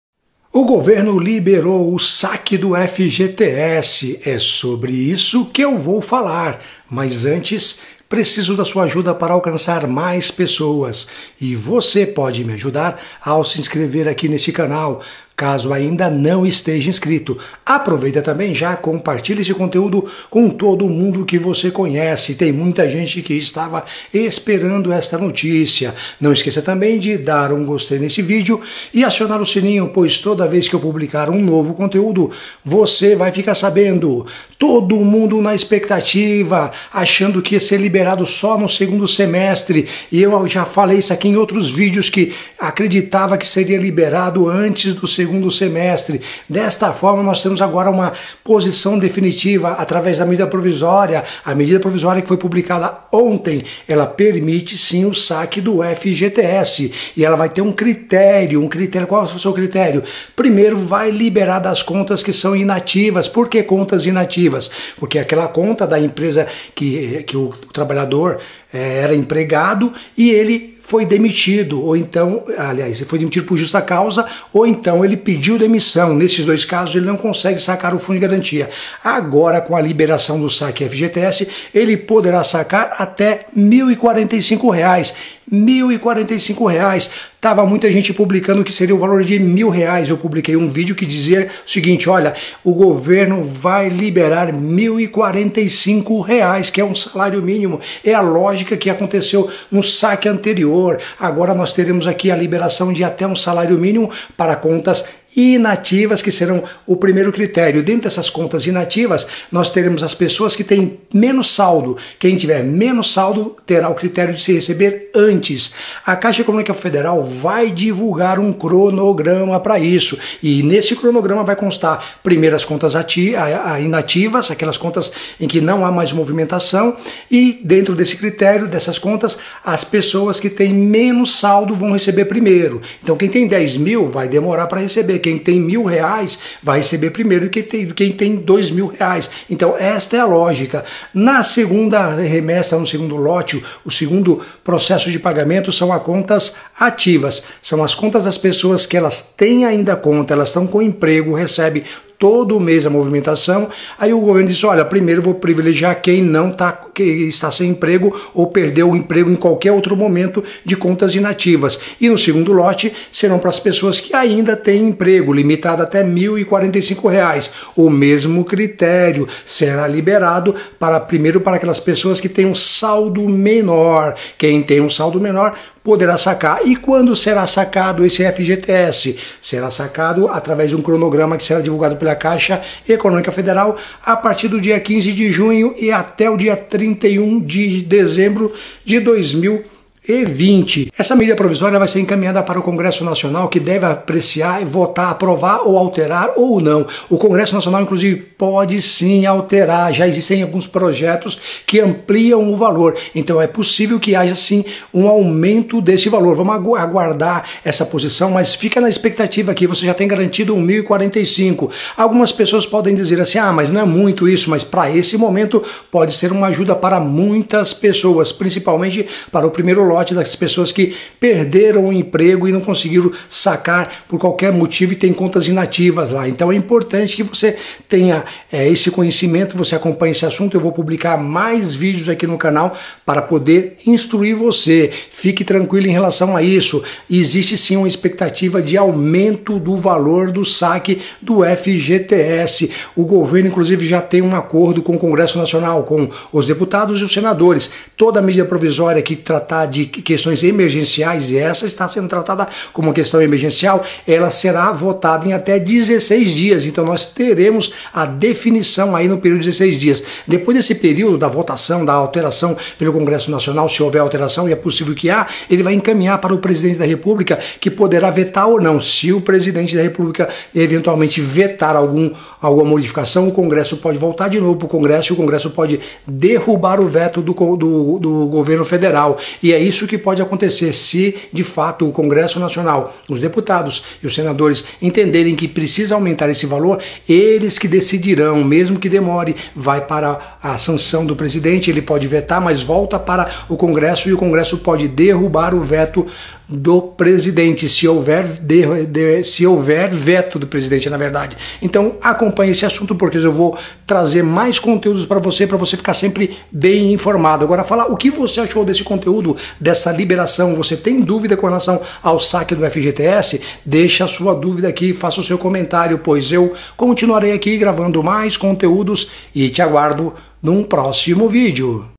FGTS LIBERADO | GOVERNO LIBEROU O SAQUE FGTS R$ 1045
Foi liberado e você poderá sacar.

Você pode ajudar a aumentar o valor do saque do FGTS, quer saber como? Assista este vídeo e entre nos link's abaixo:

Link para localizar o Deputado Federal em quem você votou e envie mensagem por e-mail perguntando o que ele pretende fazer para aumentar o valor do SAQUE do FGTS que está na Medida Provisória 946 de 2020.

Você também pode ligar para seu Deputado Federal: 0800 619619 - Ligação gratuita

Link para localizar o senador em quem você votou e envie mensagem por e-mail perguntando o que ele pretende fazer para aumentar o valor do SAQUE do FGTS que está na Medida Provisória 946 de 2020.

Telefone do Senado Federal: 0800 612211 - Ligação gratuita

Link para enviar mensagem para o Presidente da republica perguntado o que ele pretende fazer para aumentar o valor do SAQUE do FGTS que está na Medida Provisória 946 de 2020.

FGTS Liberado - MP 946 publicada R$ 1.045 por trabalhador

Quem receberá? Quem tem conta ativa e conta inativa. O pagamento será entre o dia 15 de junho à 31 de dezembro de 2020.

Começa com as contas inativas e depois a ativas.

A Caixa divulgar o calendário dos saques

Quem tem o saldo menor receberá primeiro, seguindo a ordem de conta inativa e conta ativa.

Para receber dicas sobre empreendedorismo e gestão que certamente  contribuirão com  seu negócio.